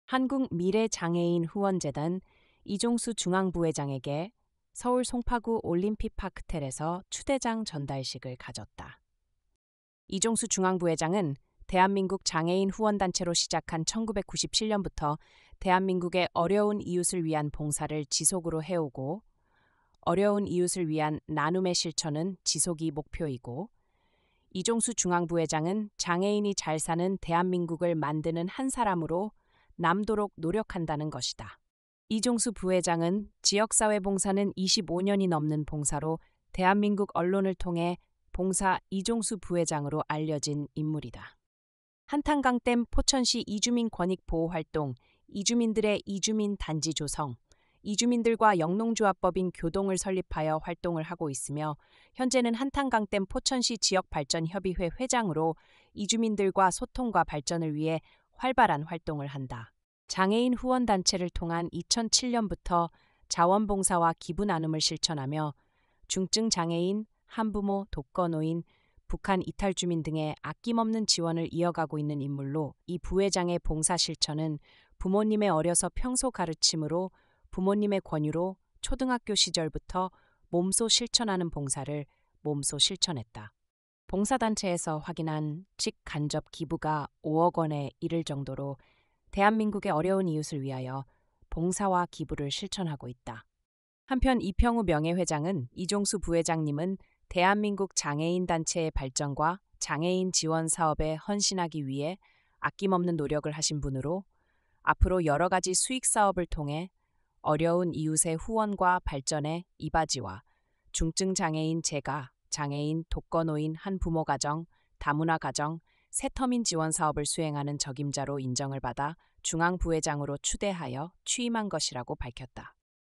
한국미래장애인후원재단은 이종수 중앙부회장 정식업무에 돌입한다고 밝혔다.
한국미래장애인후원재단(전백규 중앙회장)은 이종수 중앙부회장에게 서울 송파구 올림픽파크텔에서 추대장 전달식을 가졌다.이종수 중앙부회장은 대한민국 장애인 후원단체로 시작한 1997년부터 대한민국의 어려운 이웃을 위한 봉사를 지속으로 해오고 어려운 이웃을 위한 나눔의 실천은 지속이 목표이고, 이종수 중앙부회장은 장애인이 잘 사는 대한민국을 만드는 한 사람으로 남도록 노력한다는 것이다.이종수 부회장은 지역사회봉사는 25년이 넘는 봉사로 대한민국 언론을..본문기사 더보기. 기자